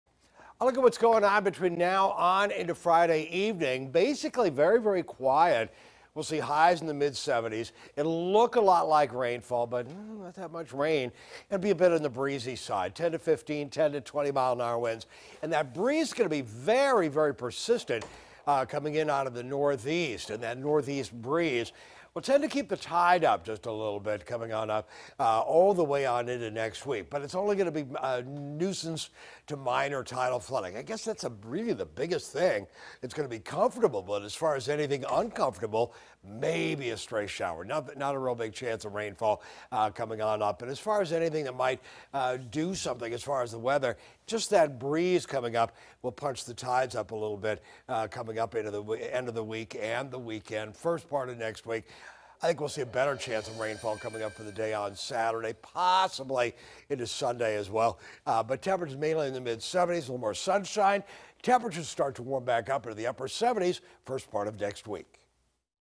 WAVY Weather Evening Update | Oct. 6, 2021
Super Doppler 10 Chief Meteorologist Don Slater has the latest weather forecast for the Hampton Roads viewing area.